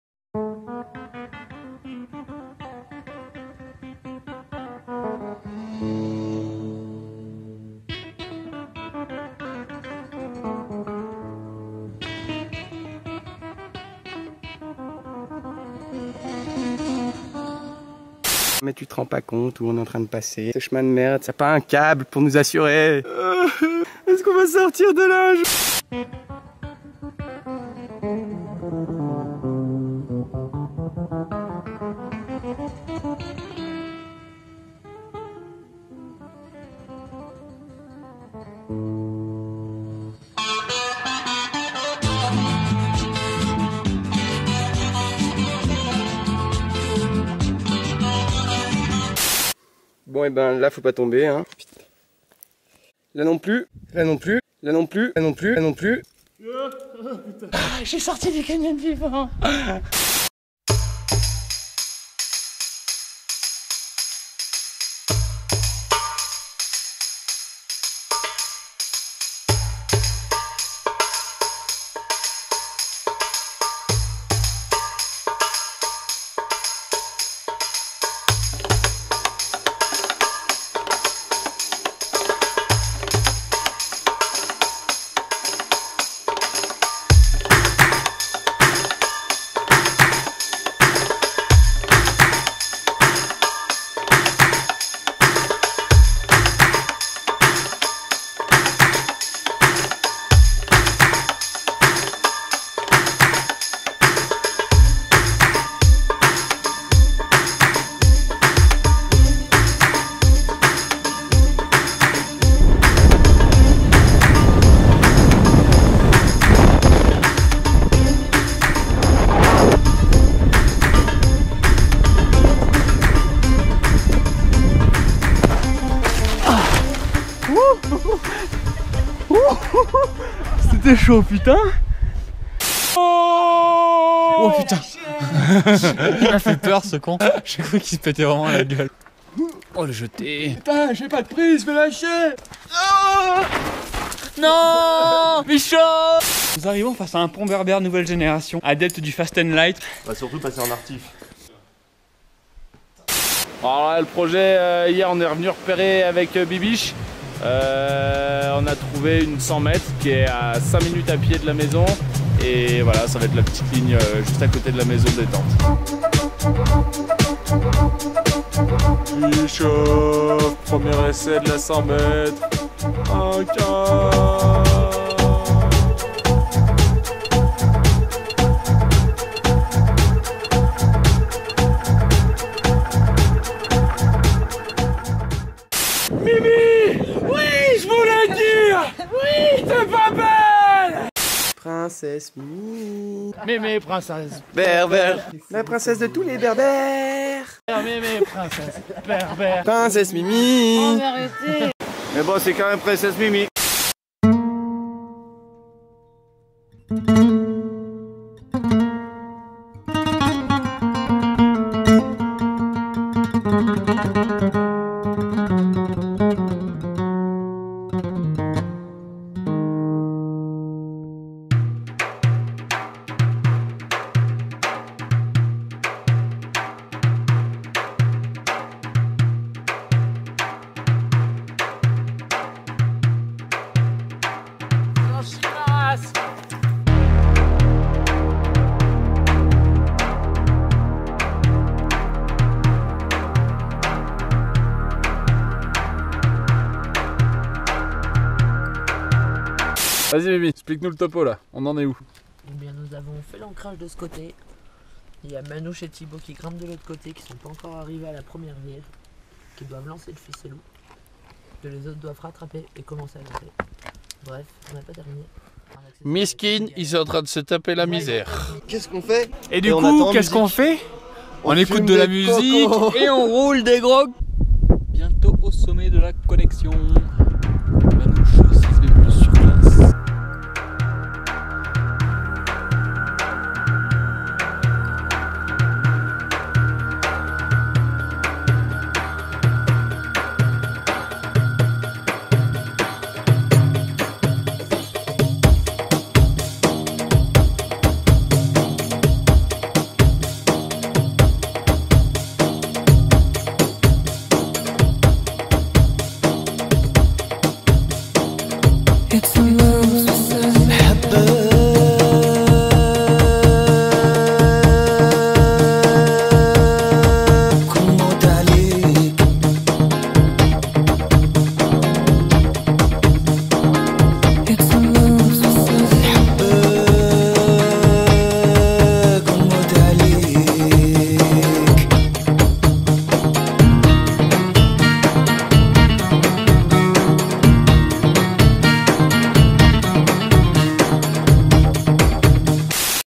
YouSlack and Friends : Raï-line, la Team Slack au Maroc
La Team Slack s'est rendu au Maroc en septembre 2016 pour aller ouvrir quelques belles lignes et partir à l'aventure chez les Berbères!